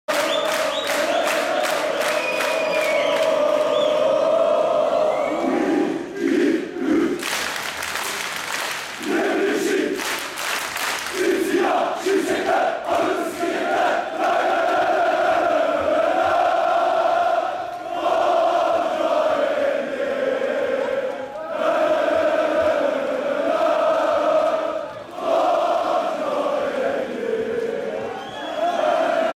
FETHİYE CADDESİNDE ÜÇLÜ(HODRİMEYDAN)
KARTAL MAÇINDAN SONRA FETHİYE CADDESİ ÜÇLÜ